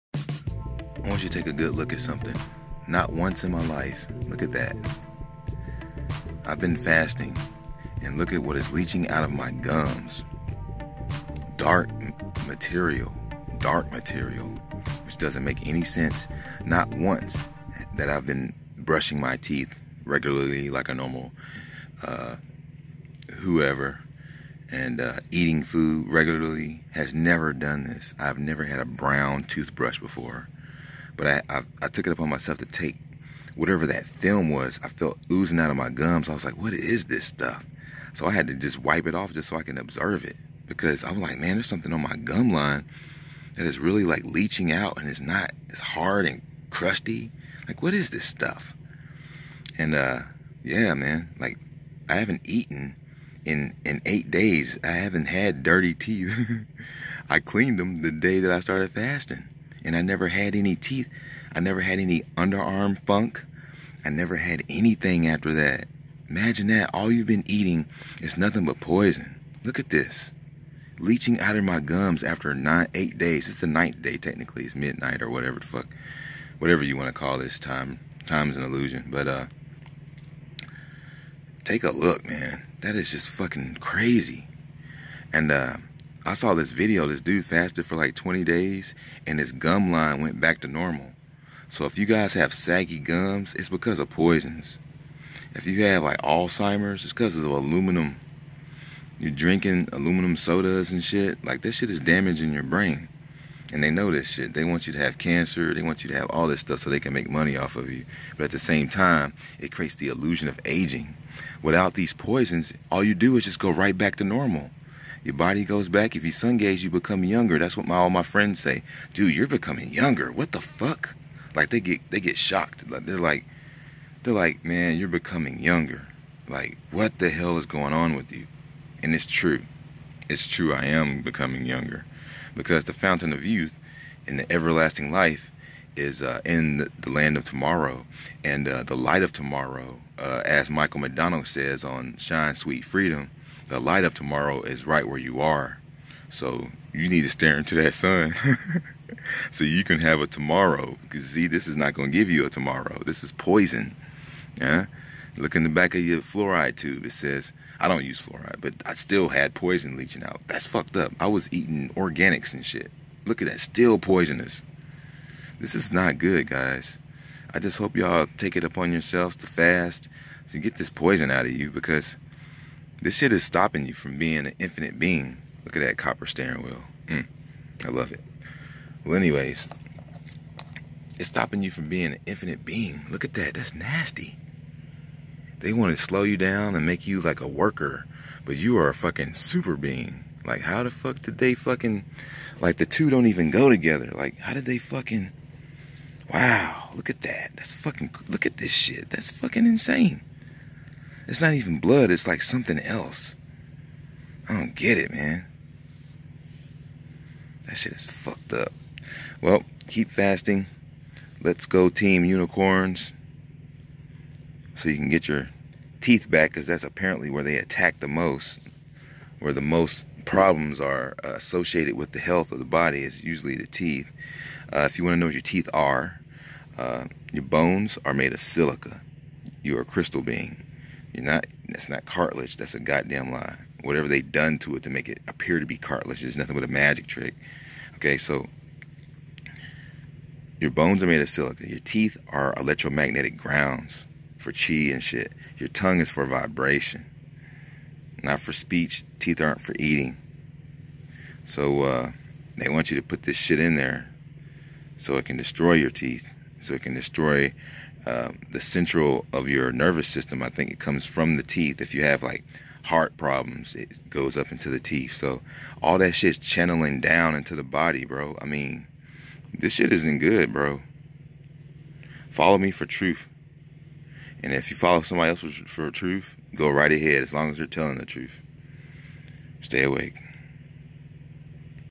What Leeched Out Of My Gums While Fasting WILL SHOCK YOU!!
I'm done with food for good now. The poisons cant stay if you dont feed them.